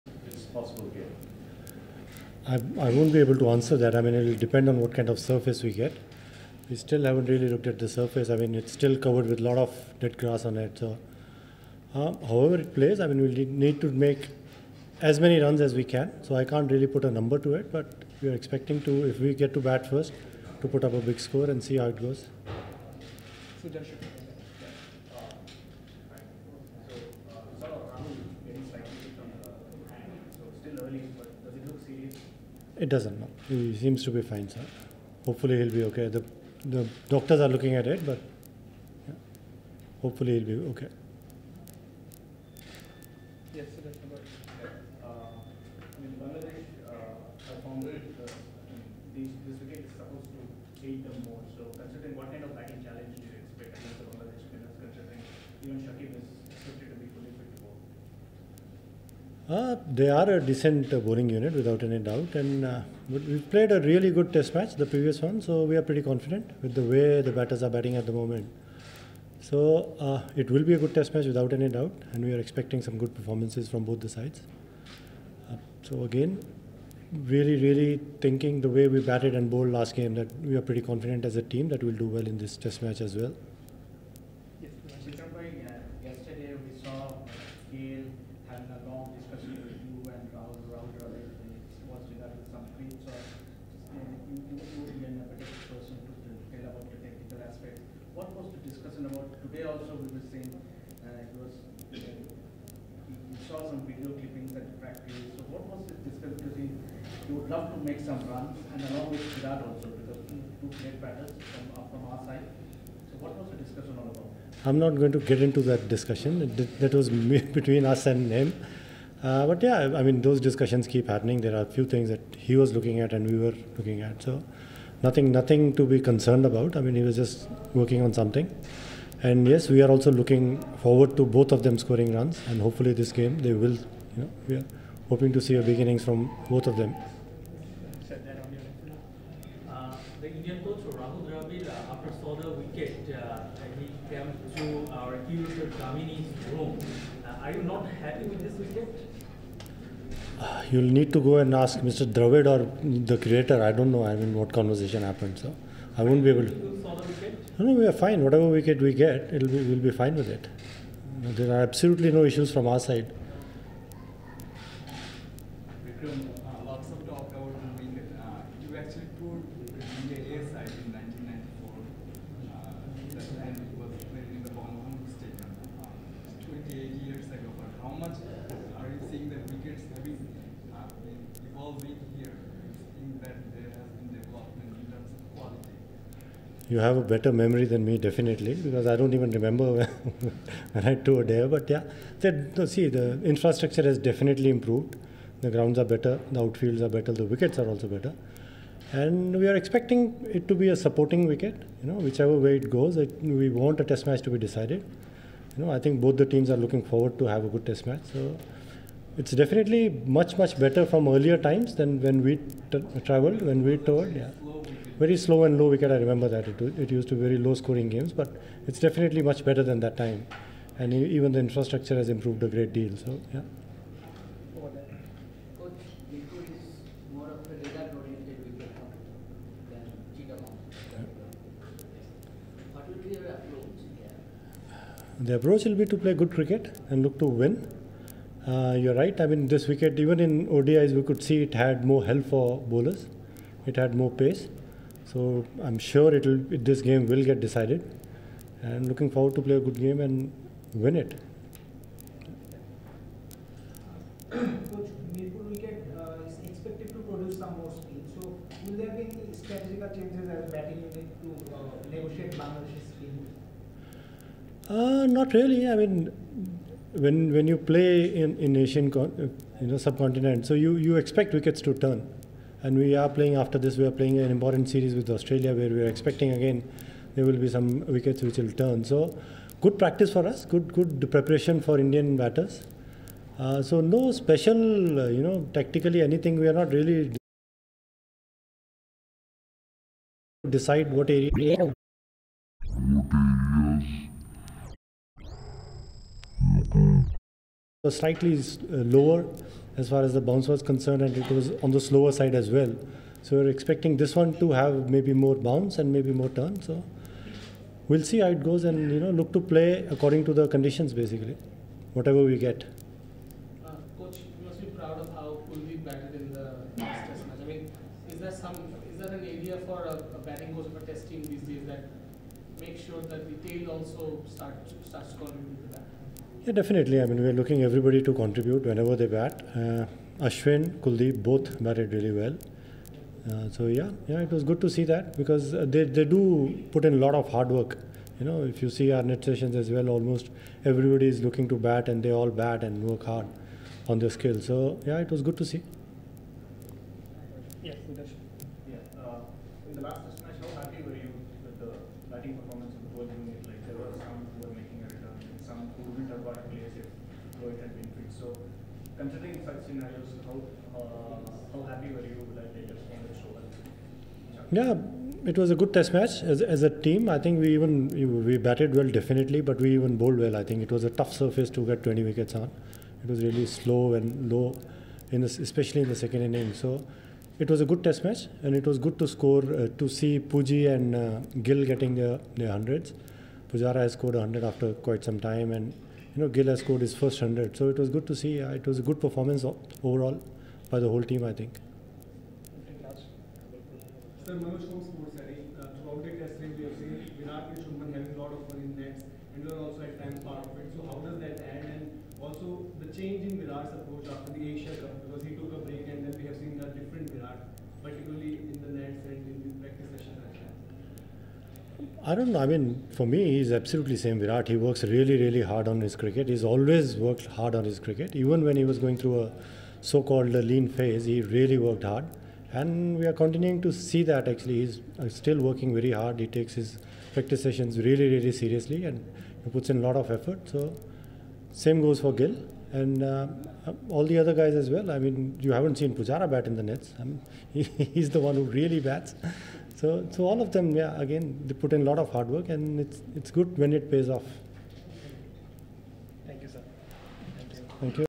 “Bangladesh Tour A Good Preparation For Australia Series”- Vikram Rathour
Indian Cricket Team is all set to lock horns against Bangladesh Cricket Team for the 2nd Test Match at Dhaka.